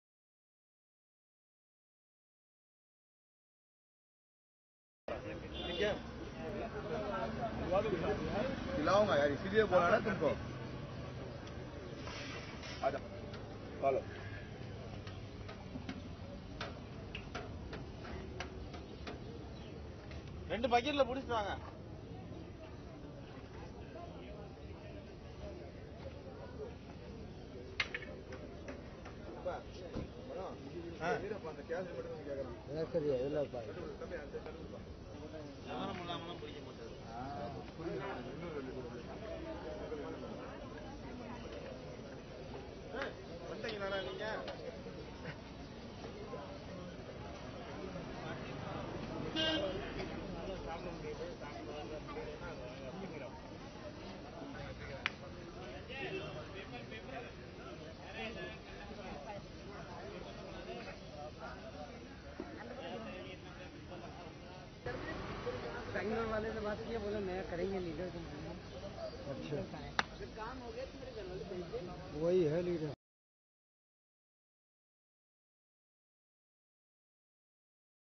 Tamil Nadu farmers continue their agitation at Jantar Mantar - ANI
New Delhi, Apr 12 (ANI): Farmers of Tamil Nadu continued their strike at the Jantar Mantar on Wednesday. The protesting farmers have warned of continuing with the stir until the crisis in the state is resolved and they are given relief package by the Centre. The farmers have been demanding that their loans from nationalised banks be forgiven, fair and just prices be set for their produce besides realistic solutions to address water scarcity in the state. They have also been demanding drought relief package of Rs. 40,000 crores from the Centre since March 14.